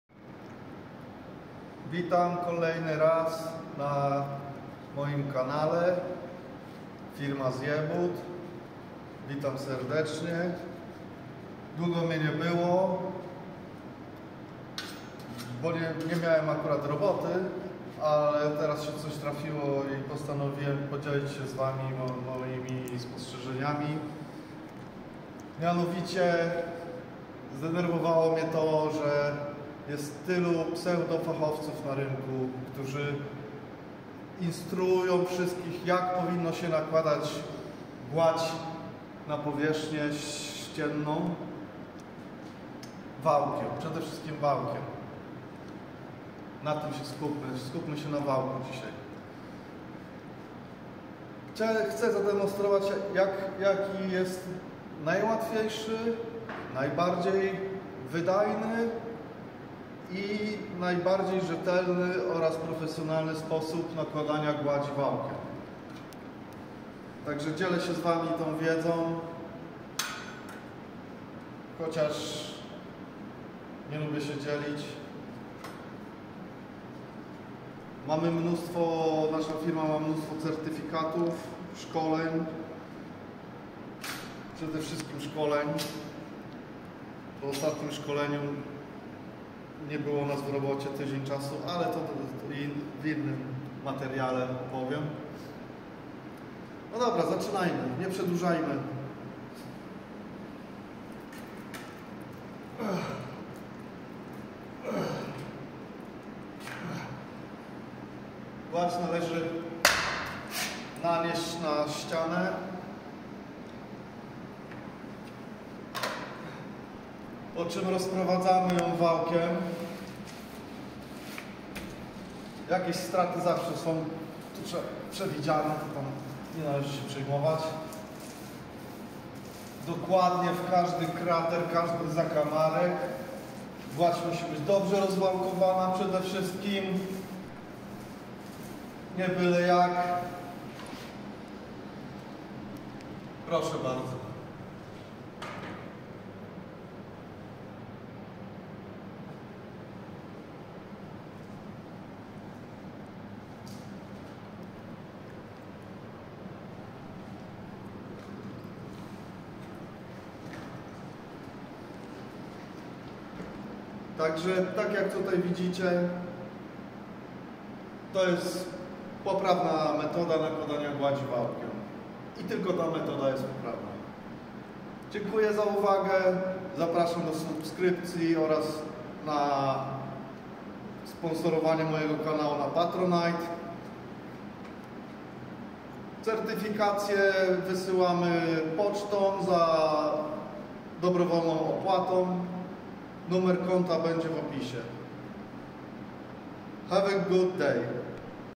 Jak prawidłowo nakładać gładź wałkiem? Jedyny prawidłowy technologicznie sposób !!
Wałek + gładź = € i $